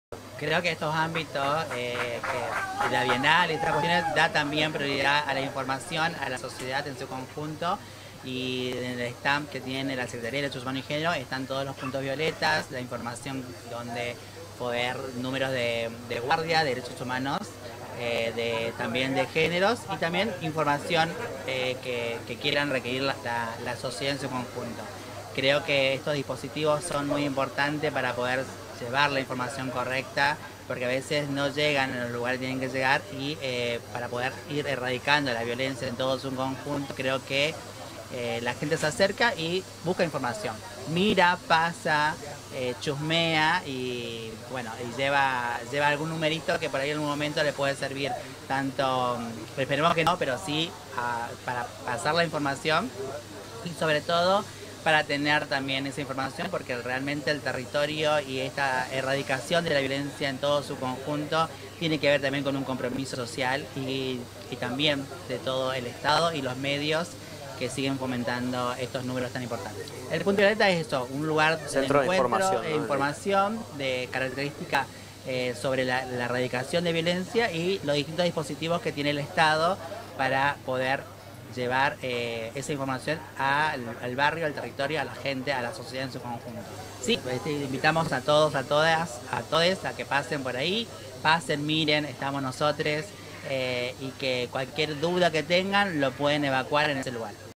BIENAL 2022: CONTINÚA EL EVENTO CULTURAL MÁS IMPORTANTE
ÚRSULA SABARESE - DIR. DE DIVERSIDAD, DISIDENCIA SEXUAL E IDENTIDAD DE GÉNERO